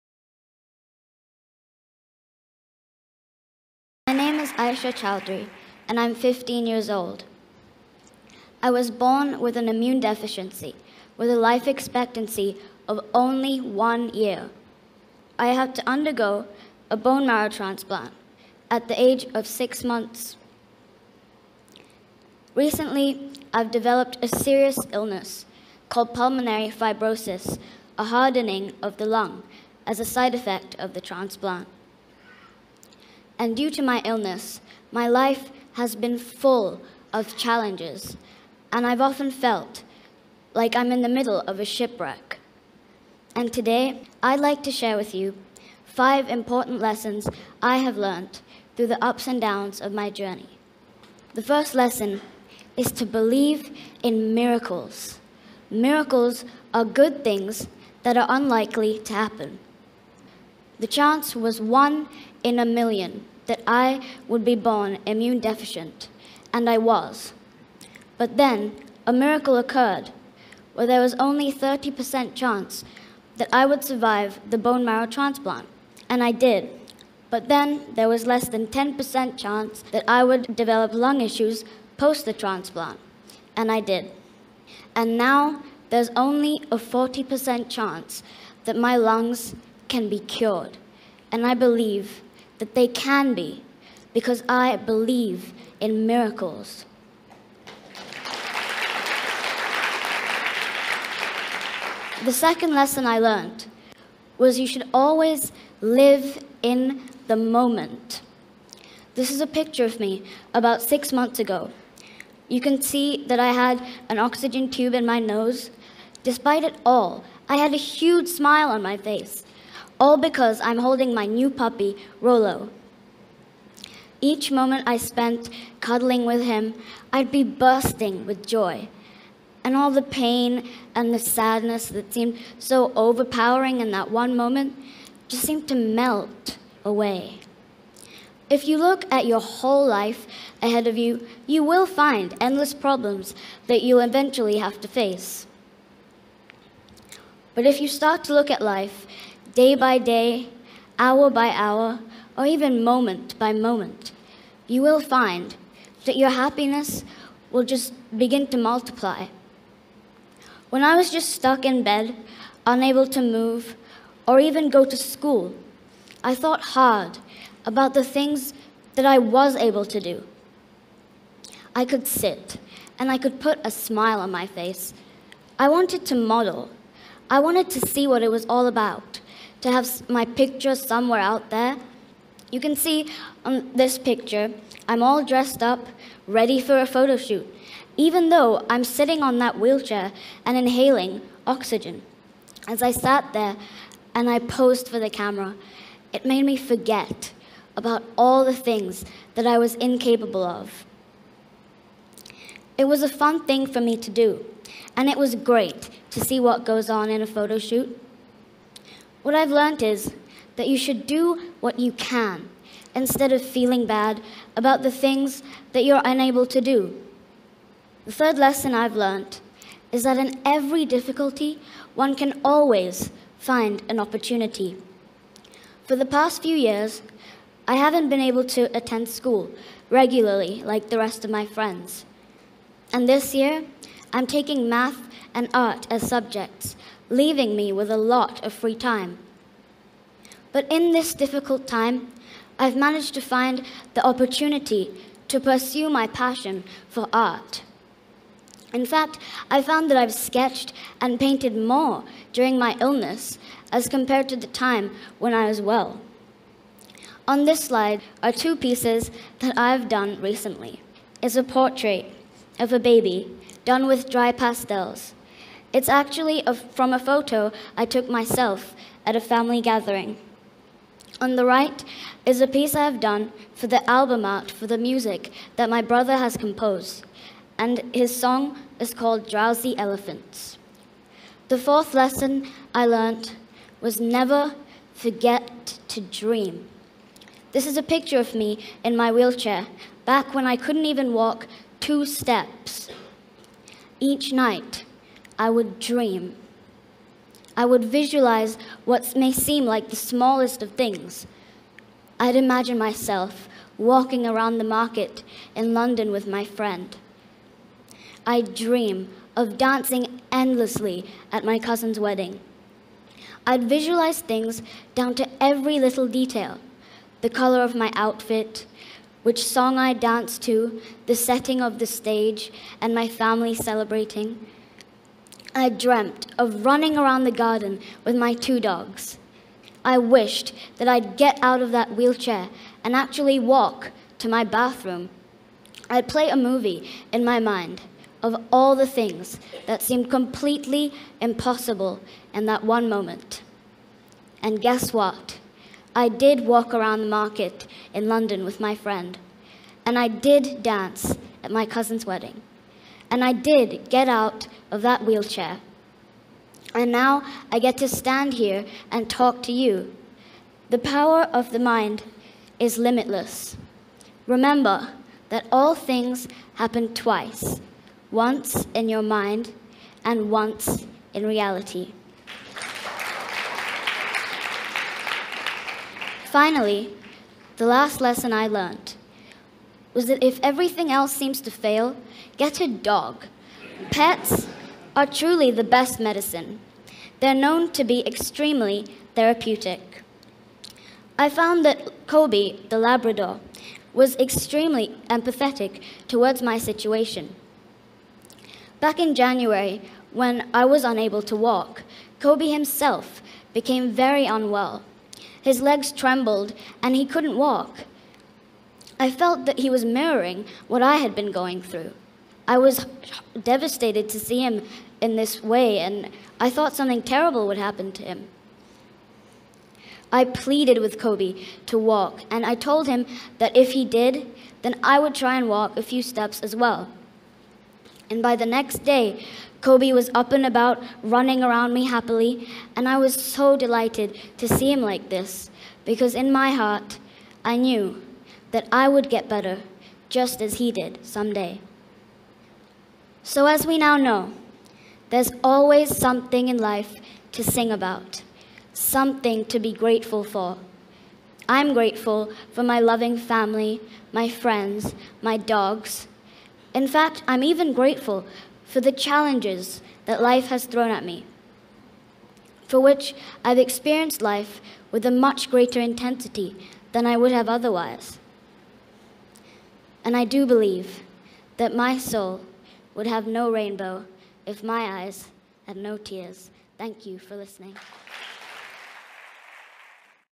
The Sky Is Pink inspired by AISHA CHAUDHARY.Her Speech few days before she passes away
Aisha Chaudhary was born with an immune deficiency disorder and overcame a predicted life expectancy of only one year to have become an accomplished artist today. Despite a serious lung disease called Pulmonary Fibrosis, her persistent optimism, extraordinary maturity in the face of impossible odds, and calm perspective on life's challenges have been an inspiration to many.

Aisha Chaudhary sadly passed away on the morning of January 24th, 2015 after battling an immune deficiency disorder and Pulmonary Fibrosis throughout her life.Bollywood movie The Sky is Pink is inspired by her story.